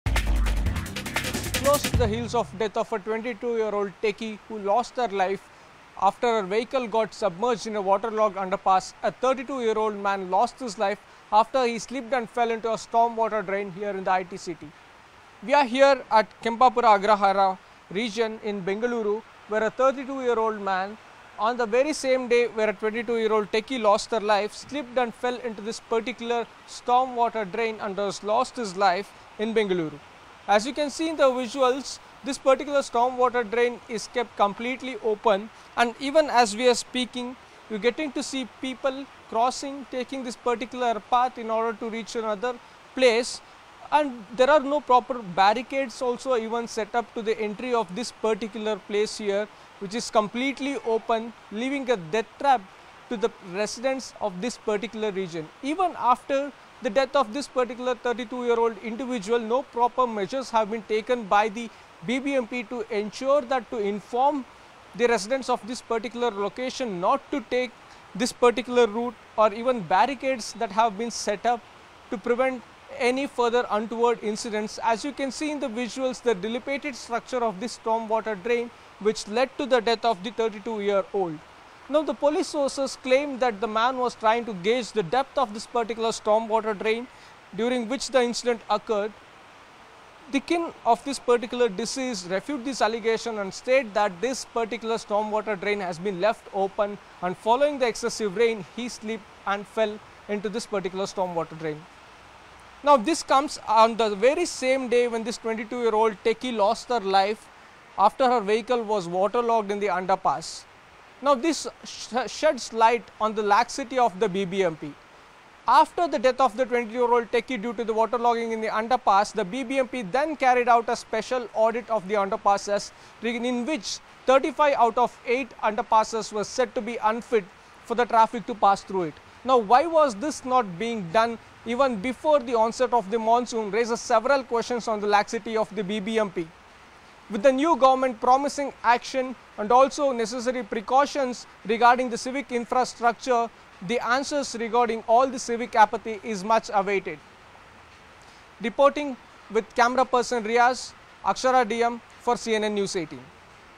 Karnataka News | Civic Apathy In Bengaluru | Bengaluru Rains | English News | News18 Exclusive
The heavy rain that lashed the city on Sunday claimed another life. The body of a man, who was washed away in an open stormwater drain around 4 pm, was found around 7 pm in the rajakaluve at Byatarayanapura. However, the incident came to light on Monday.

The victim, Lokesh R (31), was a housekeeping staffer at GT Mall and was living with his mother and two siblings at KP Agrahara.

n18oc_exclusive